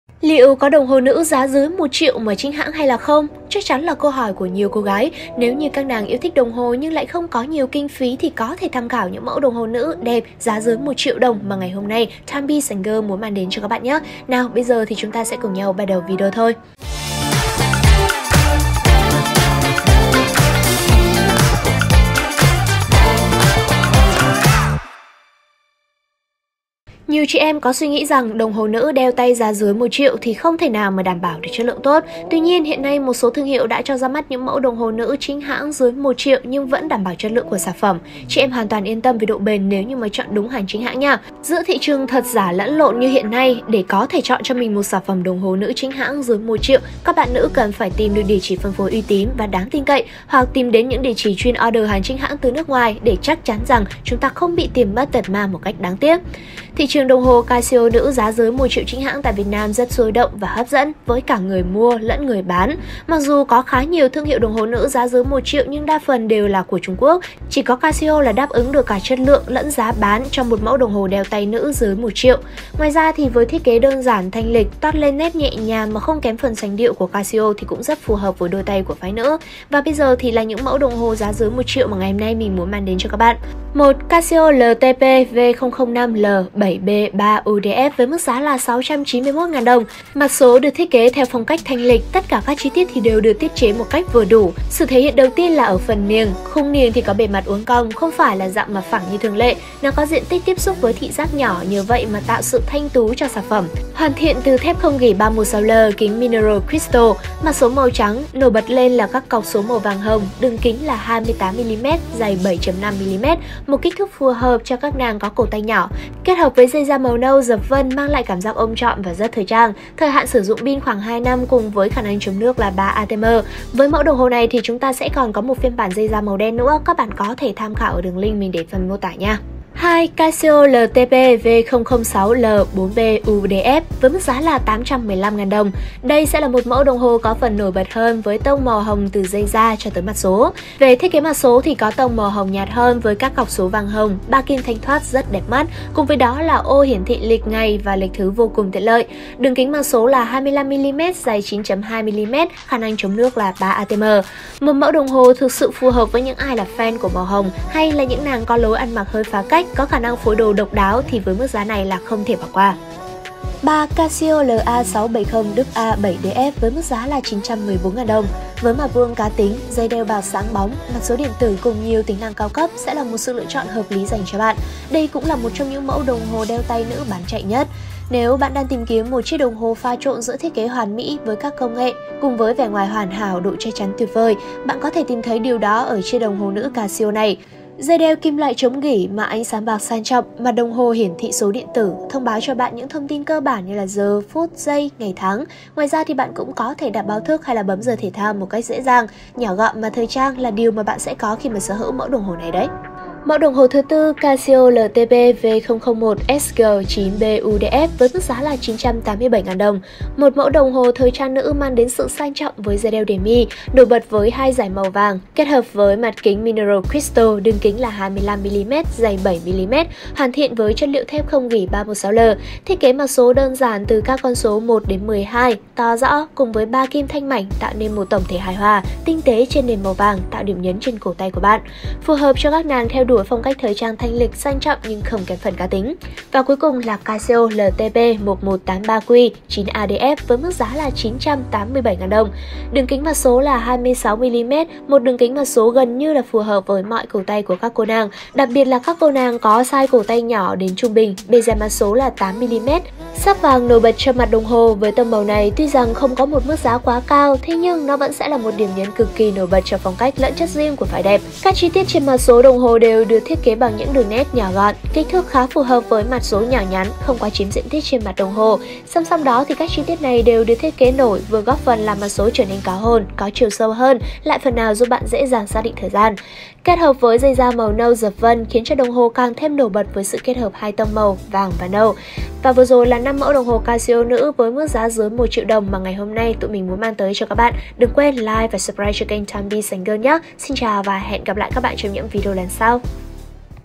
Đồng hồ Casio nữ dưới 1 triệu đồng nên mua nhất I Top đồng hồ
Liệu có đồng hồ nữ giá dưới 1 triệu chính hãng không, chắc chắn là câu hỏi của nhiều cô gái. Nếu các nàng yêu thích đồng hồ nhưng không có nhiều kinh phí thì có thể tham khảo những mẫu đồng hồ nữ đẹp giá dưới 1 triệu đồng trong video ngày hôm nay nhé!

00:00 Intro
00:26 Giới thiệu chung
hị trường đồng hồ casio nữ giá dưới 1 triệu chính hãng tại Việt Nam rất sôi động và hấp dẫn với cả người bán đồng hồ và người mua. Mặc dù có khá nhiều thương hiệu đồng hồ nữ giá dưới 1 triệu nhưng đa phần đều là của Trung Quốc. Chỉ có Casio là đáp ứng được cả chất lượng và giá cả cho mẫu đồng hồ đeo tay nữ giá dưới 1 triệu.

01:23 Casio LTP-V005L-7B3UDF
Mặt số được thiết kế theo phong cách thanh lịch, tất cả các chi tiết đều được tiết chế cách vừa đủ.
Sự thể hiện đầu tiên là ở phần niềng. Khung niềng có bề mặt uốn cong, không phải là dạng mặt phẳng như thường lệ. Nó có diện tích tiếp xúc với thị giác nhỏ, nhờ vậy mà tạo sự thanh tú cho sản phẩm.

02:10 CASIO LTP-V006L-4BUDF
Đây sẽ là mẫu đồng hồ có phần nổi bật hơn với tông màu hồng từ dây da cho tới mặt số, Về thiết kế mặt số có tông màu hồng nhạt hơn, với các cọc số vàng hồng, 3 kim thanh thoát rất đẹp mắt, cùng với đó là ô hiển thị lịch ngày và thứ vô cùng tiện lợi. Đường kính mặt số 25mm dày 9.2mm, khả năng chống nước là 3 ATM

02:43 CASIO LA670WA-7DF
Với mặt vuông cá tính, dây đeo bạc sáng bóng, mặt số điện tử cùng nhiều tính năng cao cấp sẽ là một sự lựa chọn hợp lý cho bạn, đây cũng là một trong những mẫu đồng hồ đeo tay nữ bán chạy nhất.
Nếu bạn đang tìm kiếm một chiếc đồng hồ pha trộn giữa thiết kế hoàn mỹ với các công nghệ mới cùng vẻ ngoài hoàn hảo và độ chắc chắn tuyệt vời. Bạn có thể tìm thấy điều đó ở chiếc đồng hồ nữ Casio này

03:25 Casio LTP-V001SG-9BUDF
Một mẫu đồng hồ thời trang nữ mang đến sự sang trọng với dây đeo demi, nổi bật với hai dải màu vàng, kết hợp với mặt kính mineral đường kính 25mm dày 7mm. Hoàn thiện chất liệu thép không gỉ 316L. Thiết kế mặt số đơn giản với các số từ 1 - 12 to rõ cùng 3 kim thanh mảnh

03:56 Casio LTP-1183Q-9ADF
Đường kính mặt số: 26 mm, đường kính mặt số này gần như phù hợp với mọi cổ tay của các cô nàng, đặc biệt là các cô nàng có size cổ tay từ nhỏ đến trung bình.
Bề dày mặt số: 8 mm
Sắc vàng nổi bật cho mặt đồng hồ. Với tông màu này, tuy đồng hồ Casio LTP-1183Q-9ADF có mức giá không quá cao thế nhưng nó vẫn sẽ là một điểm nhấn cực kỳ nổi bật cho phong cách lẫn chất riêng của phái đẹp.

♪ Hotline tư vấn bán hàng: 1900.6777